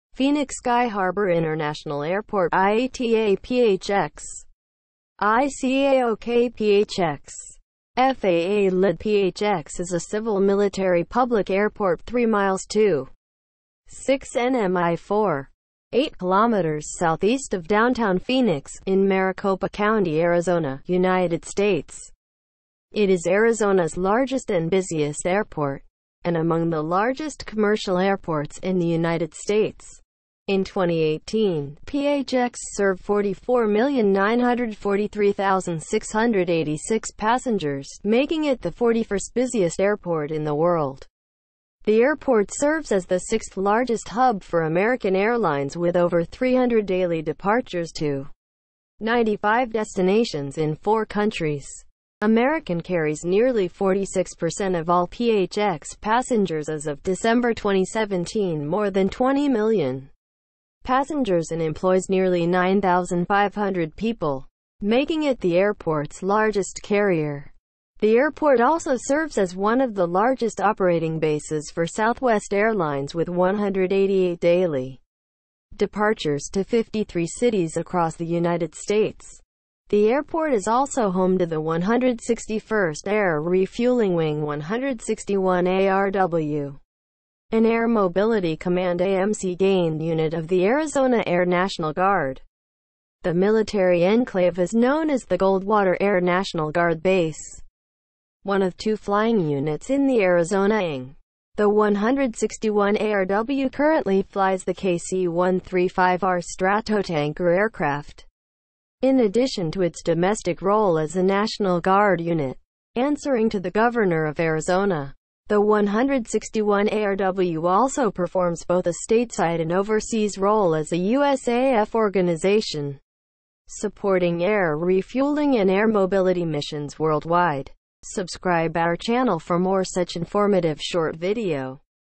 Learn about phoenix sky harbor international airport | what is sky priority
A detail information about what is sky priority. I do not own it. I just make video out of this so that some people get some help.